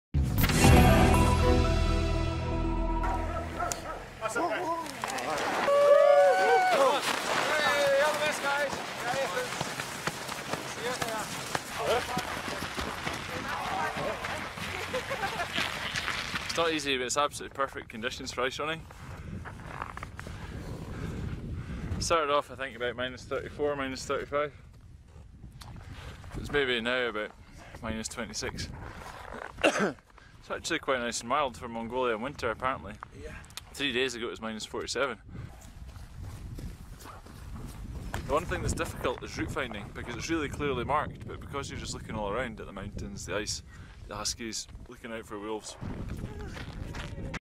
Marathon lopen in -35 graden
Een Britse hardloper heeft de eerste editie van de Genghis Khan Ice Marathon in Mongolië gewonnen. Tijdens de loop krijgen de lopers van deze run te maken met bizarre temperaturen.